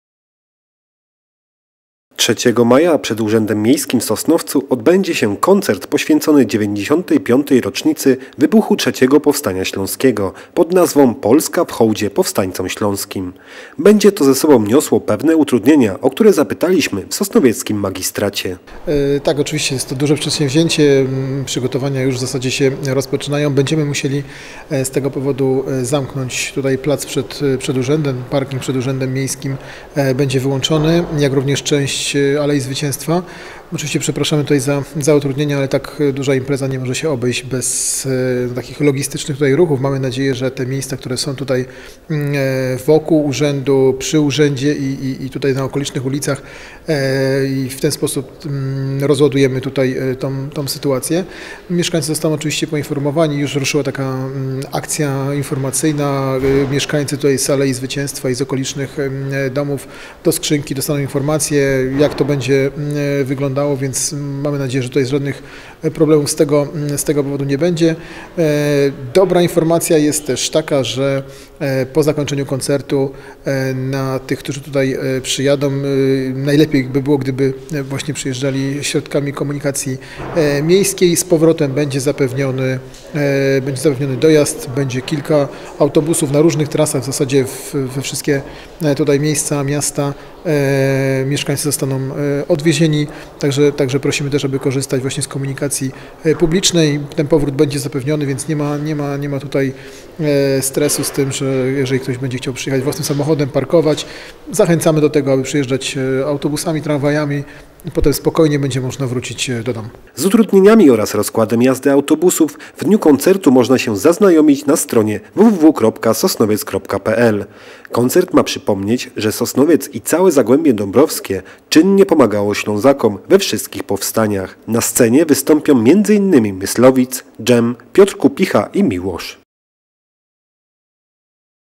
Utrudnienia związane z koncertem w hołdzie Powstańcom Śląskim
Już jutro na placu przed Urzędem Miejskim w Sosnowcu odbędzie się duży koncert w hołdzie Powstańcom Śląskim. Wiąże się to z pewnymi utrudnieniami, o które zapytaliśmy w sosnowieckim magistracie.